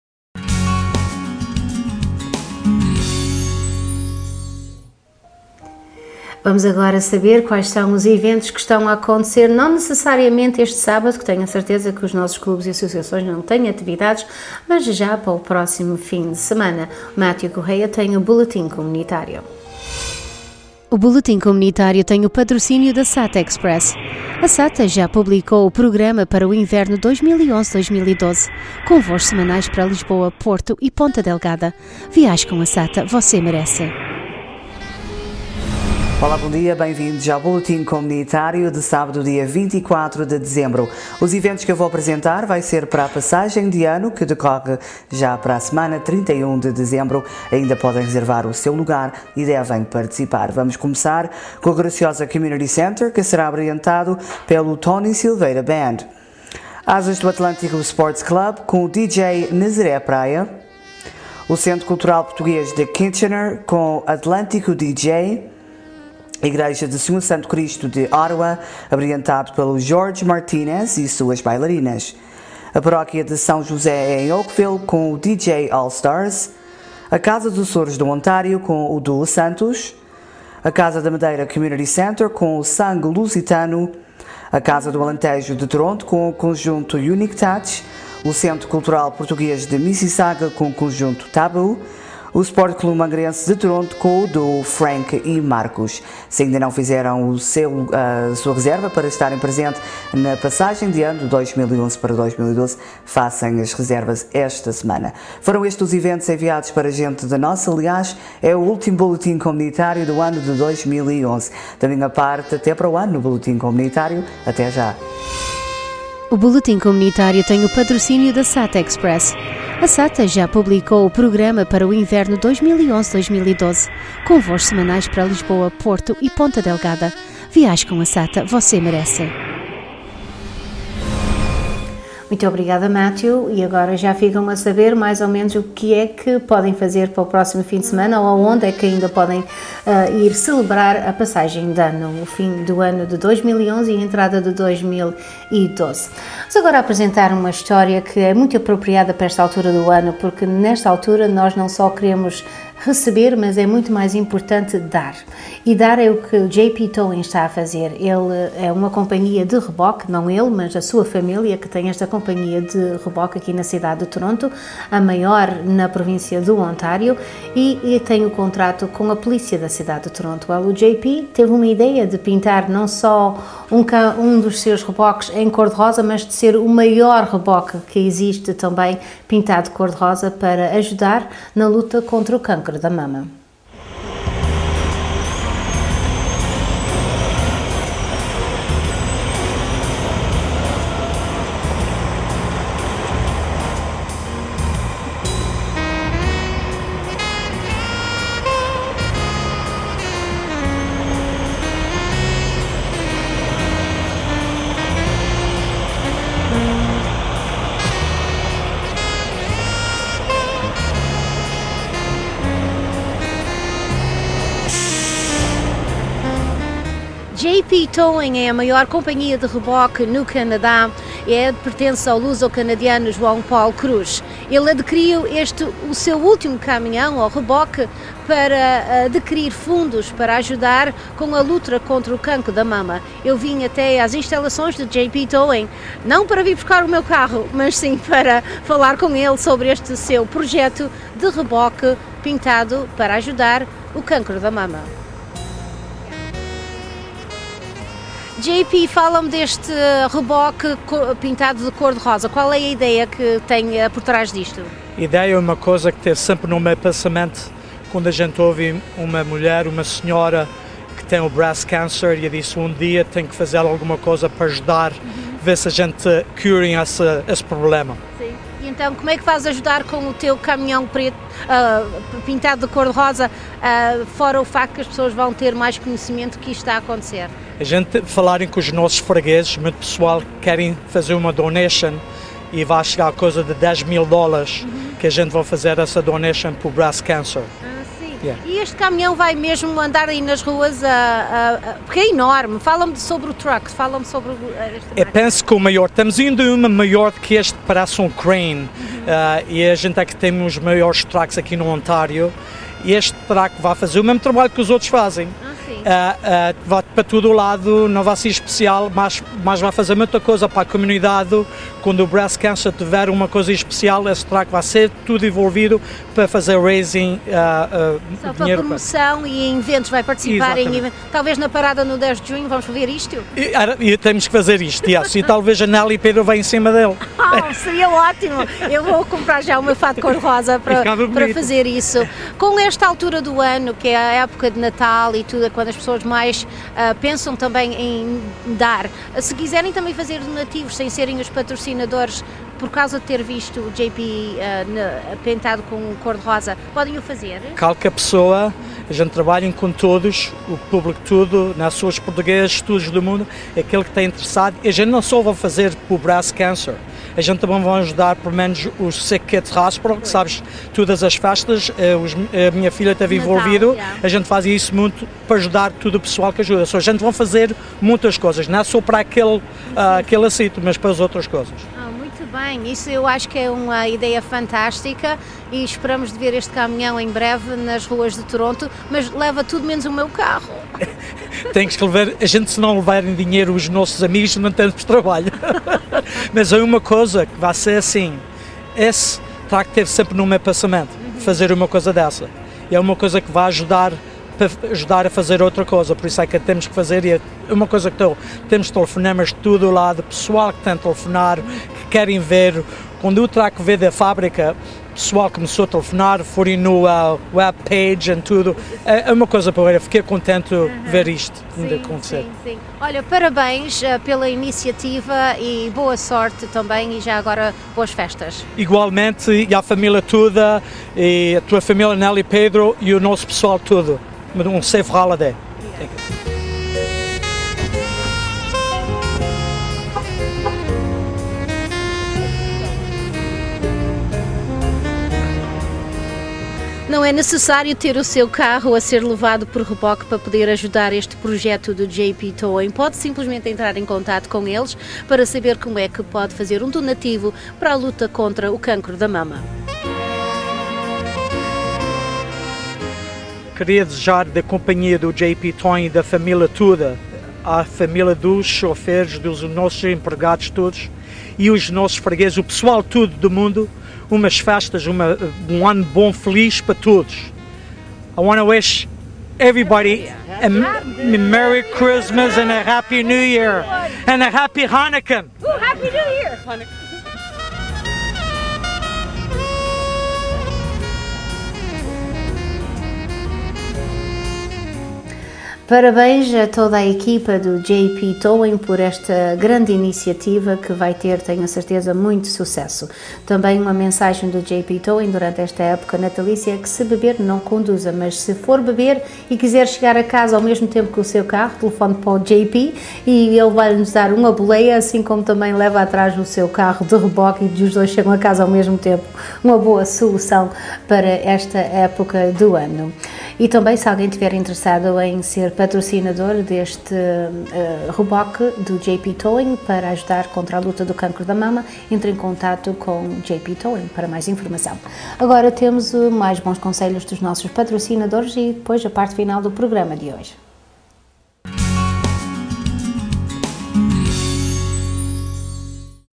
JP Towing & Pink Tow Truck - Dec24 - #5
Boletim Comunitario & conversa com John Paul Cruz da firma JP Towing & a aquisicao do novo reboque cor-de-rosa em prol da Canadian Breast Cancer Foundation - Cancro da Mama.